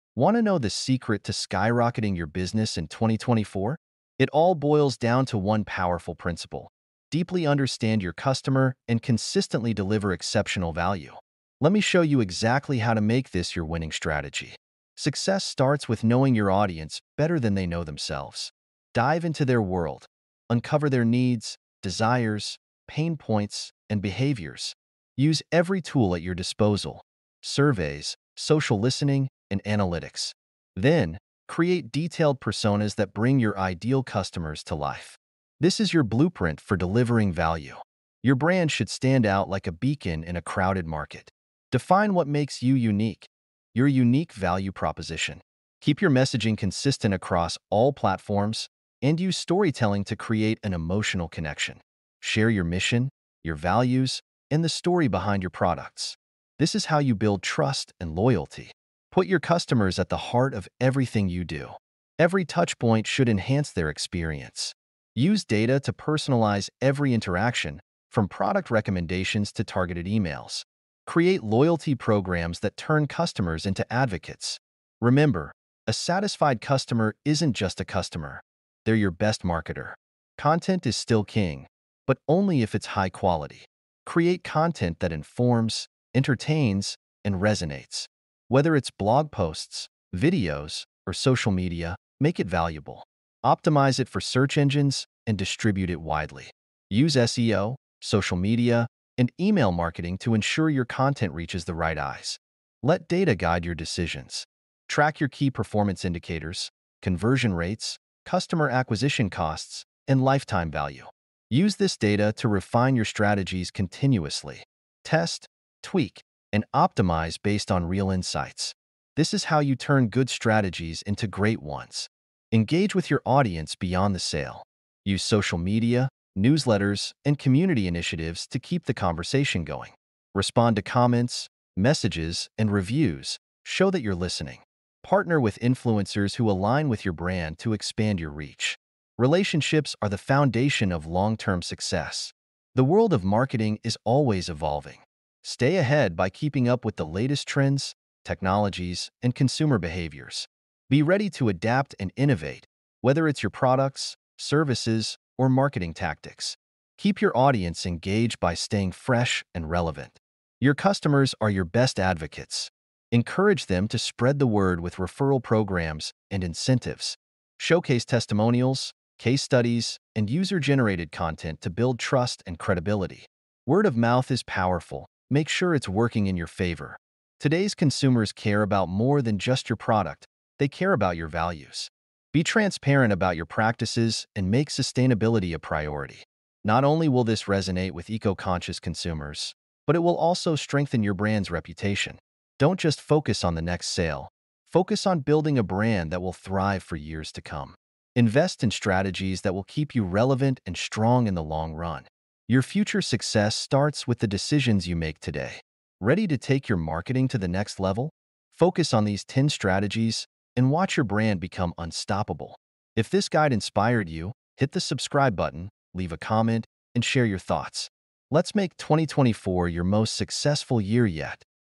7 effective marketing strategies ( Tips tracks & tactics
This video talks about marketing strategies to unlock business success in 2024.

It emphasizes the importance of understanding your customers, creating high-quality content, and personalizing the customer experience. Additionally, it highlights the need to stay ahead of marketing trends, engage with customers beyond the sale, and focus on building a sustainable, long-term brand with effective marketing strategies.

Chapters:

00:00 Introduction: Skyrocket Your Business in 2024
00:05 Understand Your Customer
00:35 Define Your Unique Value Proposition
01:03 Personalize Customer Interactions
01:30 Create High-Quality Content
01:59 Data-Driven Strategy
02:28 Engage Beyond the Sale
02:55 Stay Ahead of Trends
03:25 Leverage Word of Mouth
03:56 Embrace Sustainability
04:18 Focus on Long-Term Success